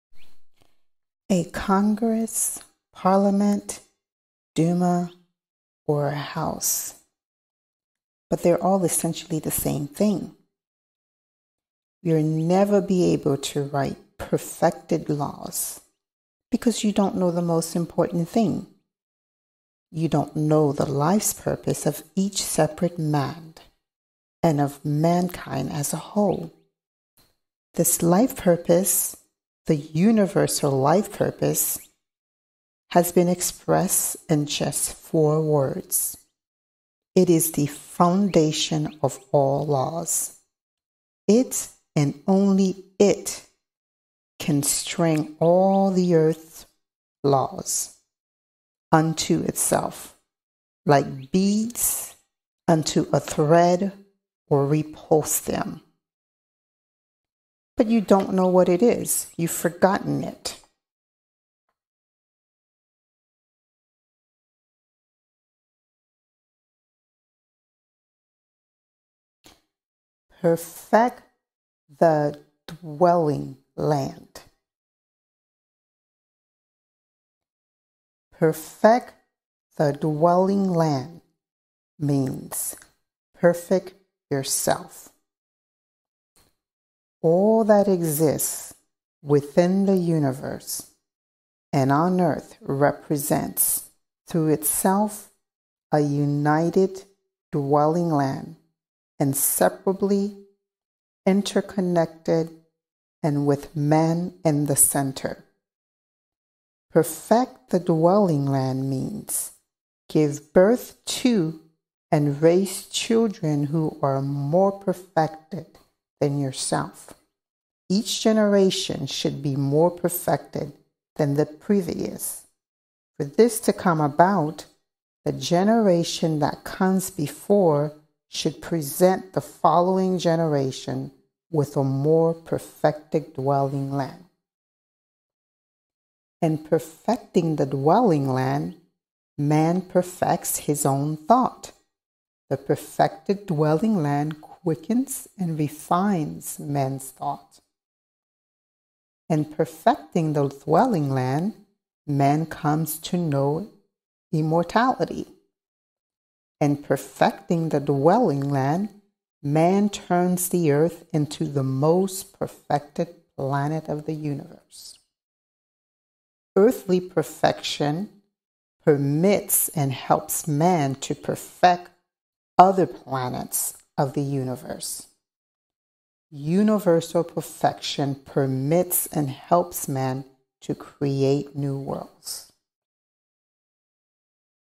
Notice to Congress- Parliament-Duma-House
Read and Narration by Empress Nefertiti

The word Man (with a capital M) is used throughout the book to refer to a human
being of any gender.

A Congress, Parliament, Duma, or a House, they’re all essentially the same thing.

You’ll never never be able to write perfected laws because you don’t know the most important thing. You don’t know the life’s purpose - the Universal life’s purpose- has been expressed in just four words. It is the foundation of all laws. It and only it, can string all the Earth’s laws onto itself, like beads onto a thread, or repulse them. But you don’t know what it is - you’ve forgotten it.

“PERFECT THE DWELLING LAND”

“Perfect the dwelling land means perfect yourself.

“All that exists within the Universe and on Earth represents through itself a united dwelling land, inseparably interconnected, and with man in the center.

“Perfect the dwelling land means give birth to and raise children who are more perfected than you yourself. Each generation should be more perfected than the previous. For this to come about, the generation that comes before should present the following generation with a more perfected dwelling land.

“In perfecting the dwelling land, man perfects his own thought. The perfected dwelling land quickens and refines man thought.

“In perfecting the dwelling land, man comes to know mortality.

“In perfecting the dwelling land, man turns the Earth into the most perfected planet of the Universe.

“Earthly perfection permits and helps man to perfect other planets of the Universe.

“Universal perfection permits and helps man to create new world.

- Darked-haired and Anastasia

From Anasta by Vladimir Megre